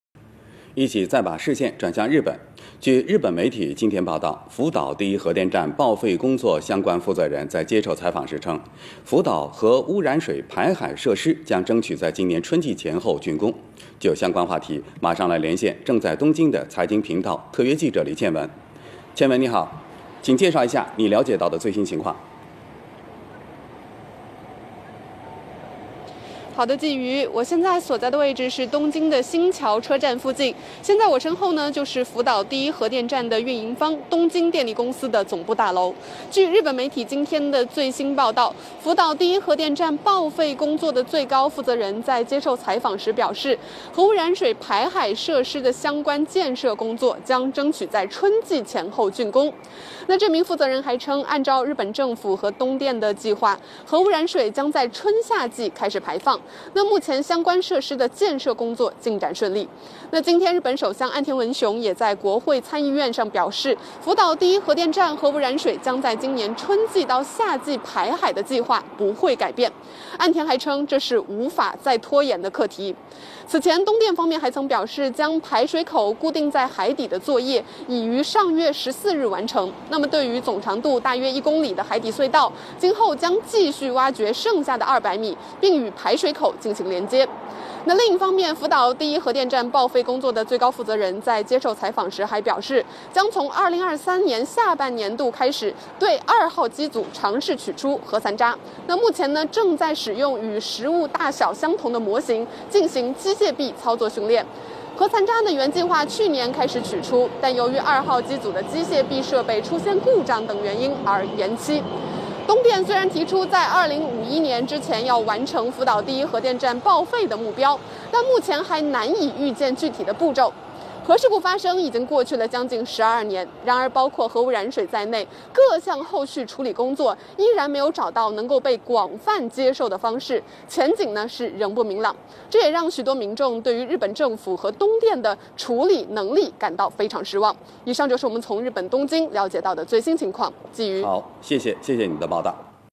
日本首相称核污水排海无法再拖延
福岛核污水排海时间不变 据日媒3日报道，福岛第一核电站报废工作最高负责人在接受采访时表示，福岛核污染水排海设施将争取在春季前后竣工。这名负责人称，核污染水将在春夏季开始排放。日本首相岸田文雄表示，核污水排海计划不会改变，这是无法再拖延的课题。